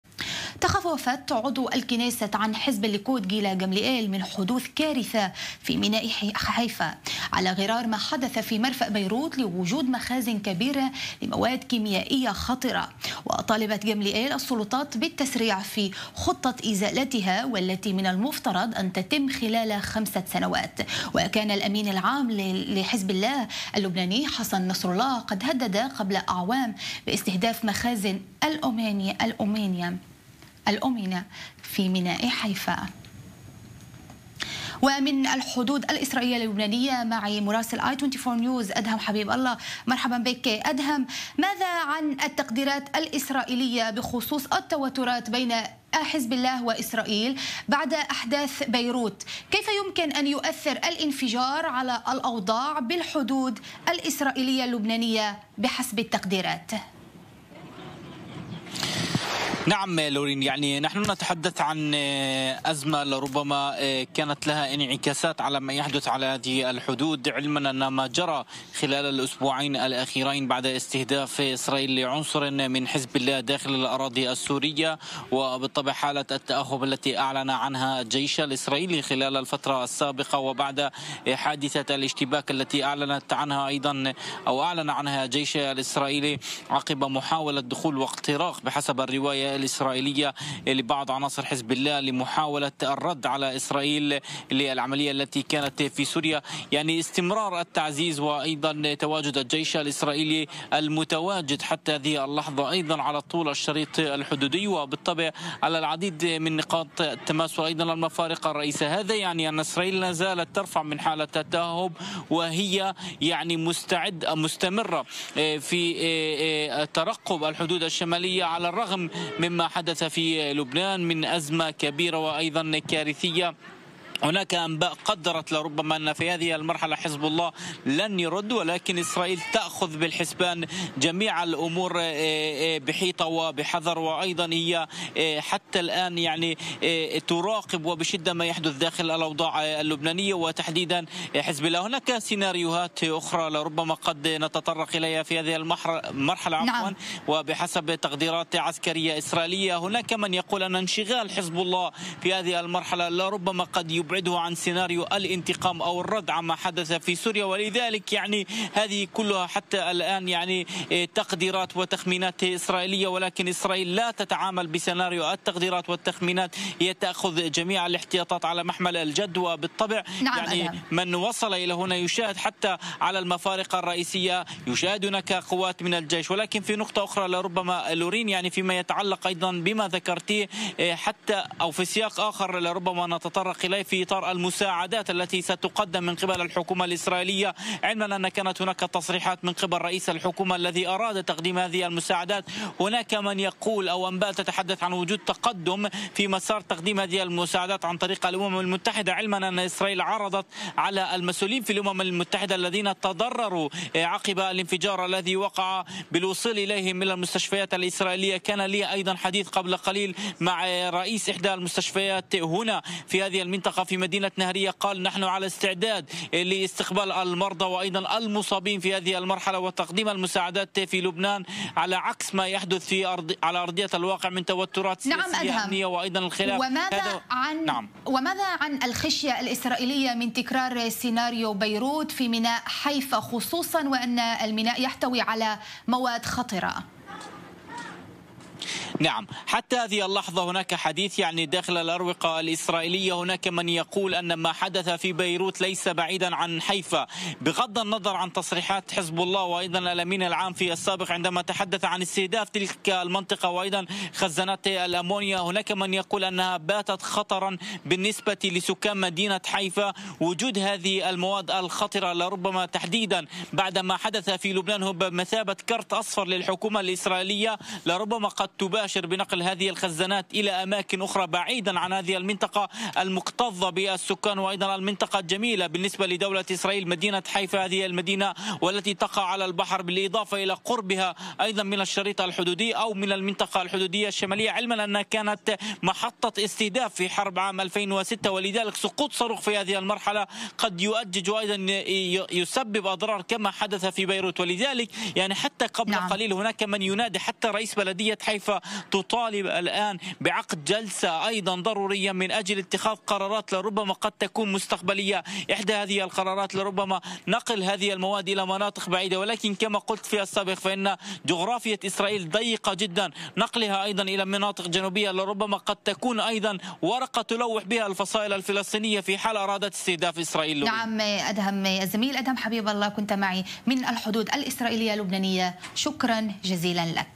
مخاوف إسرائيلية من "كارثة" في حيفا على غرار بيروت
لمتابعة موقع i24news بالعربية:
يخشى إسرائيليون، حدوث "كارثة" في ميناء حيفا، على غرار ما حدث في مرفأ بيروت، لوجود مخازن كبيرة لمواد كيمائية "خطرة".